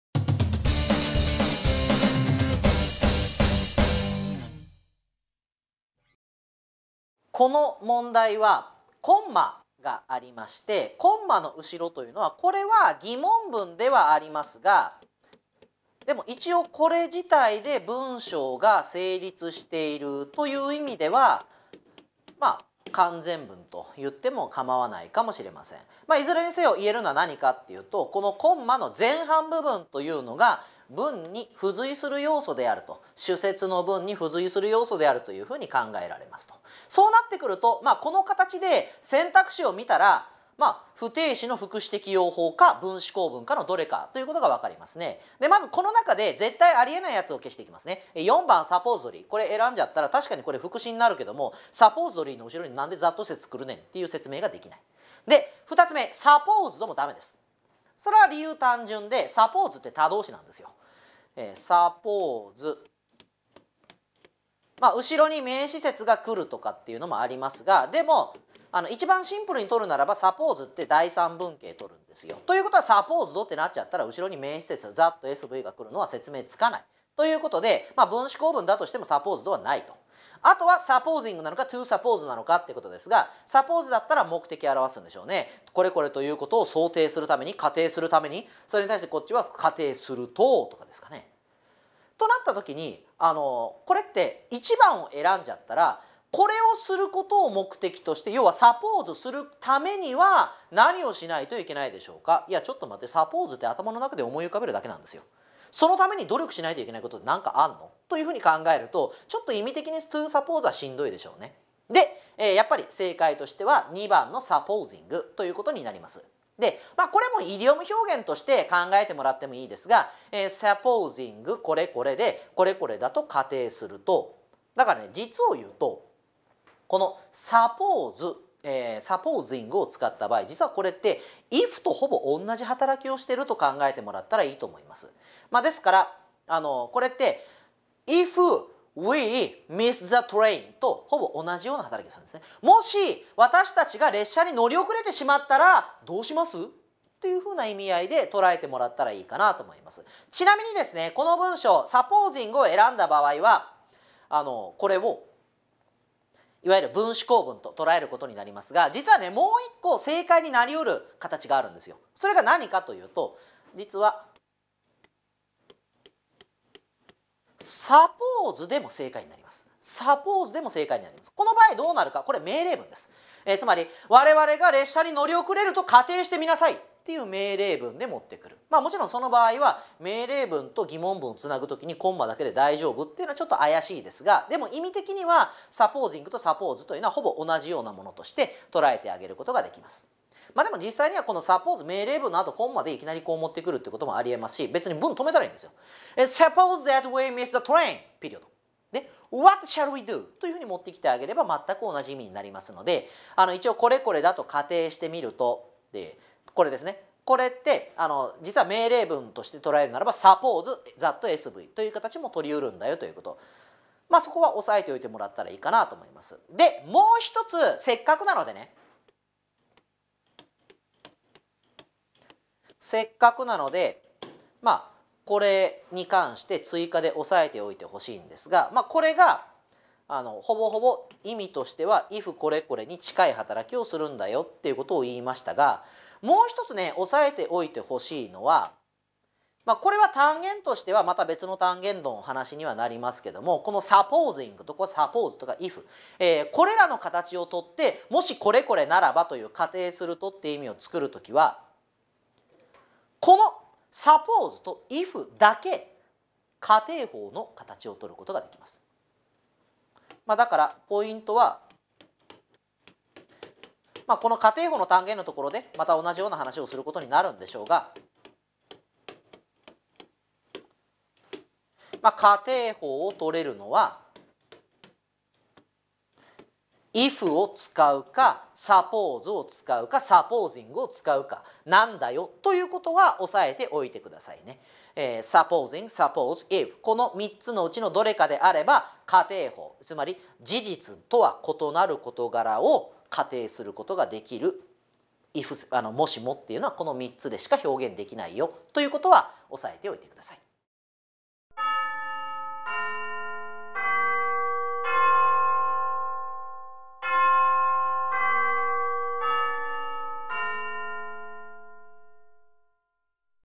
マイドリ099_分詞
次の問題を解説しています。挑戦したい方は制限時間30秒でトライしてみて下さい！
適語選択
(     ) that we miss the train, what shall we do?
1. To suppose　2. Supposing　3. Supposed　4. Supposedly